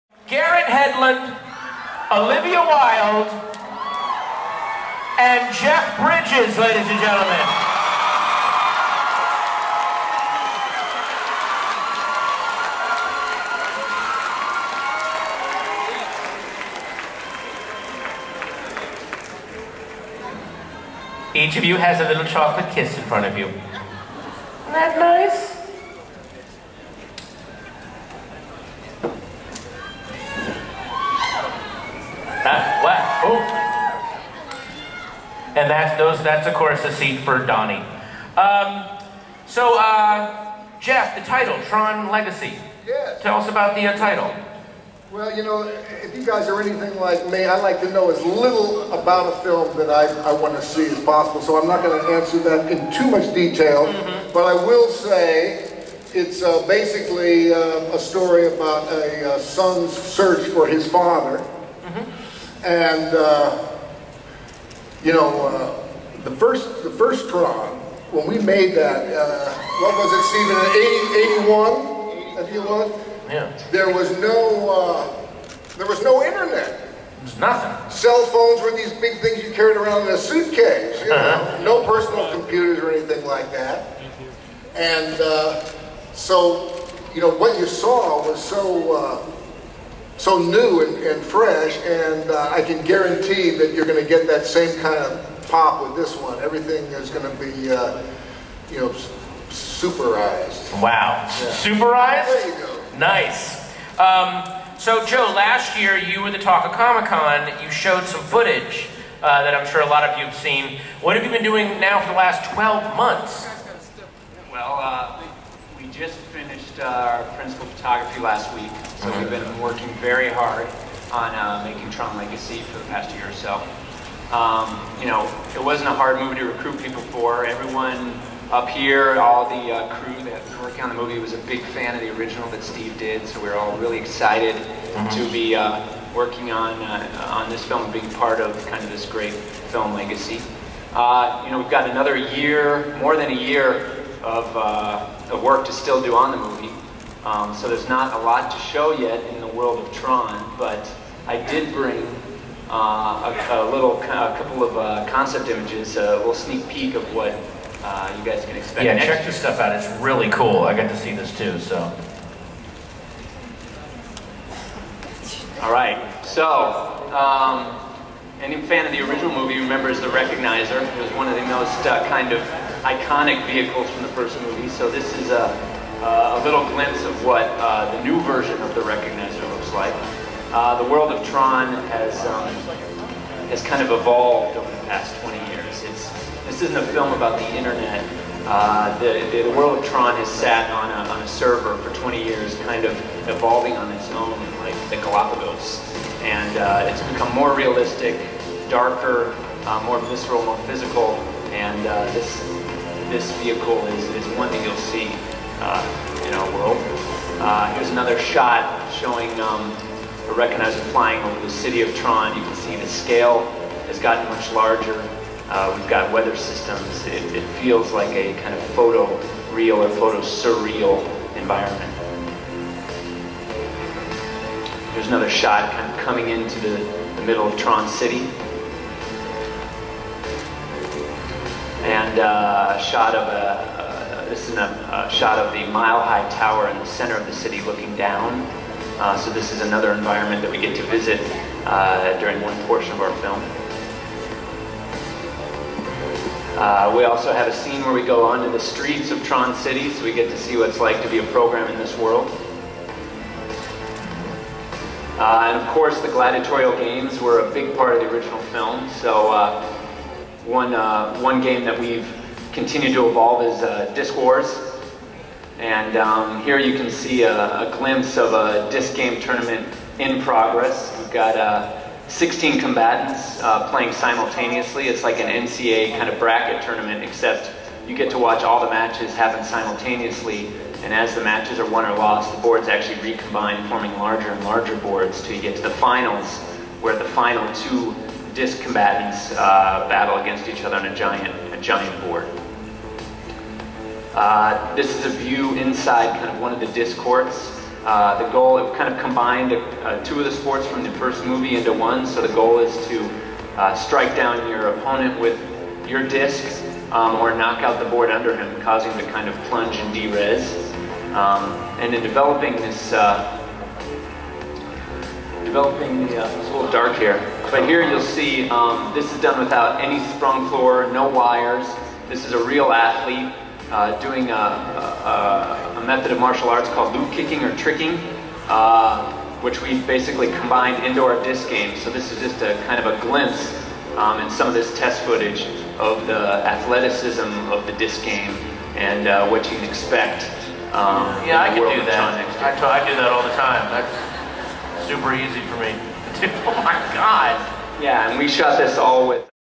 Comic Con 2009: Tron Legacy Part 1
Walt Disney Pictures presents a discussion and Q&A of their new film, "Tron Legacy," a sequel to the groundbreaking cult-classic, "Tron," starring Jeff Bridges. Patton Oswalt discusses the film with stars Jeff Bridges, Olivia Wilde, Garrett Hedlund, director Joseph Kosinski, and writer Steven Lisberger, who also directed the original "Tron."